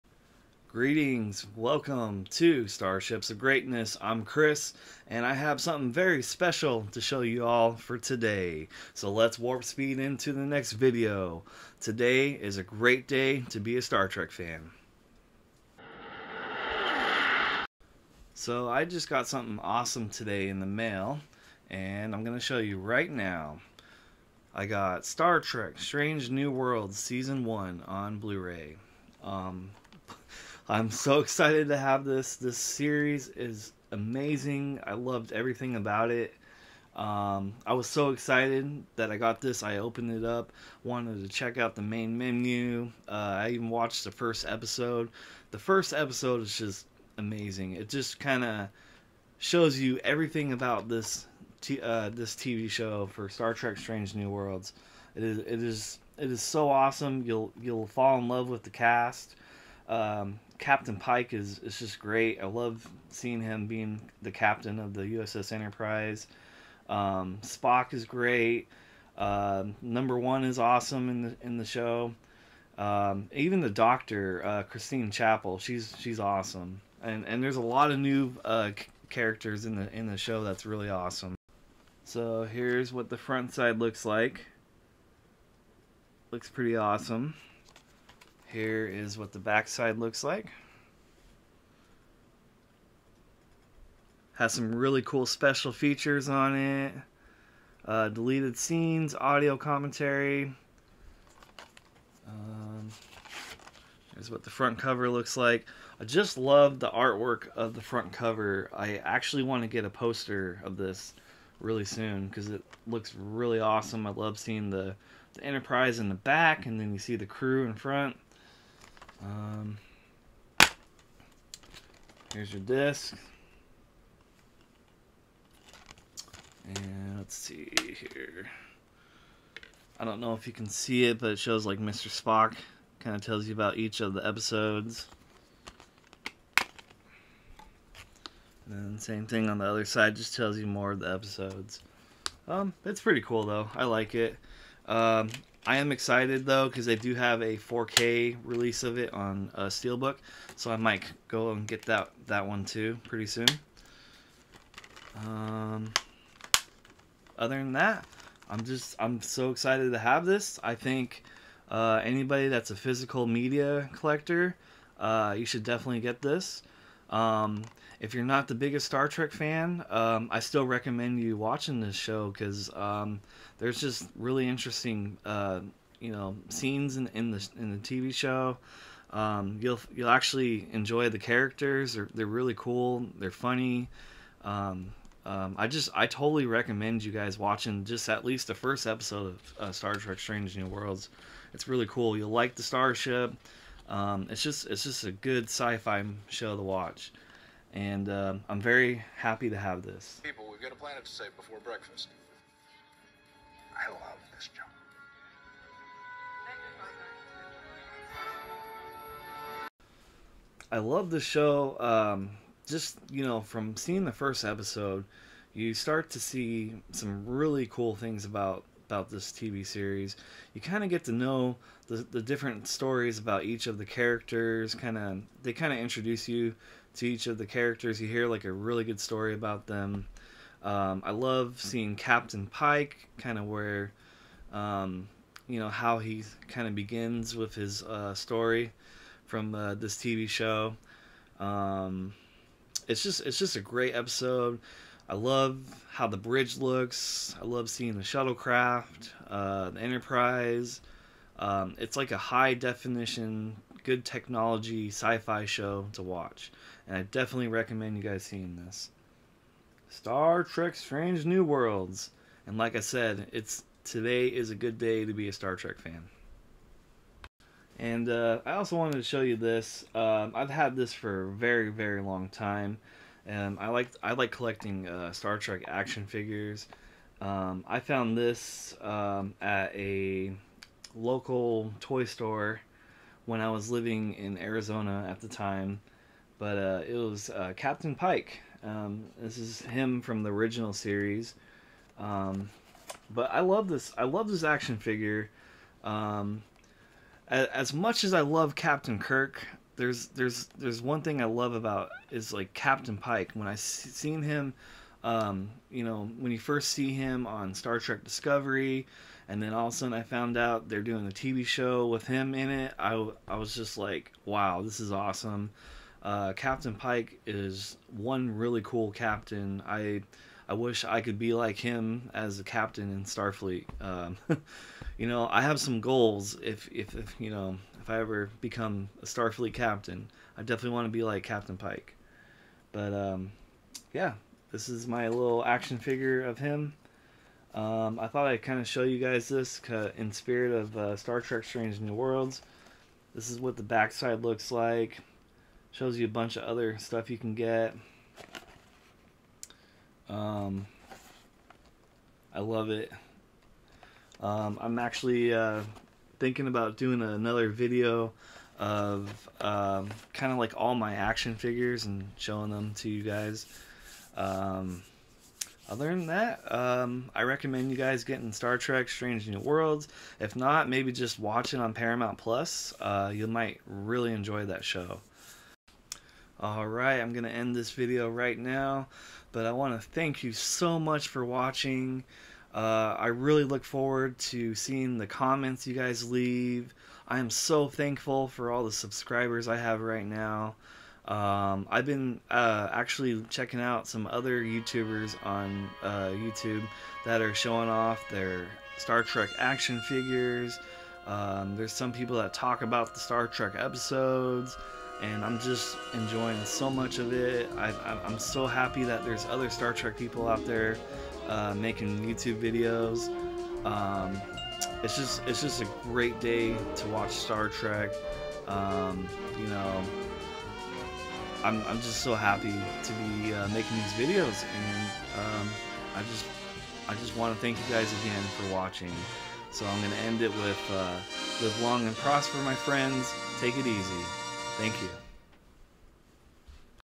Star Trek Strange New Worlds Blu-Ray
I finally got my new Star Trek Strange New Worlds Blu-Ray I'm so excited to share with you all.